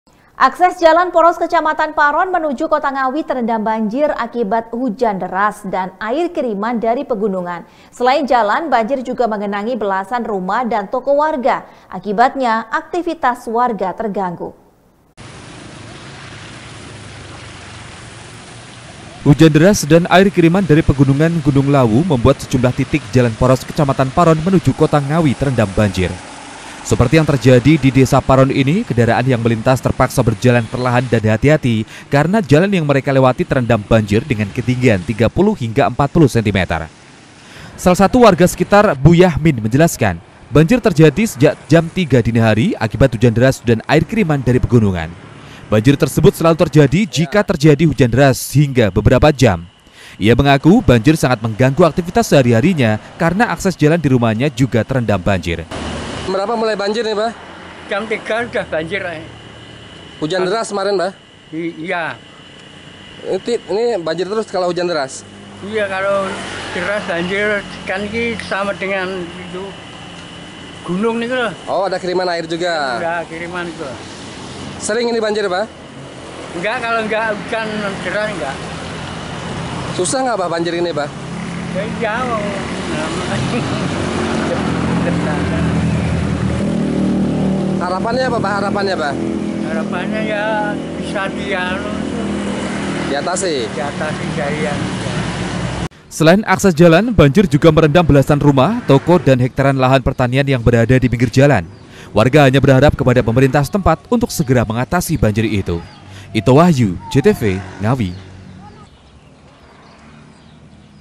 NGAWI - Jalan Poros Kecamatan Terendam Banjir, Aktivitas Warga Terganggu
Akses jalan poros kecamatan paron menuju kota Ngawi terendam banjir akibat hujan deras dan air kiriman dari pengunungan. Selain jalan banjir juga menggenangi belasan rumah dan toko warga. Akibatnya aktivitas warga terganggu..
Office : jl. Sarimulya no 47 Rejomulyo, Kartoharjo, Kota Madiun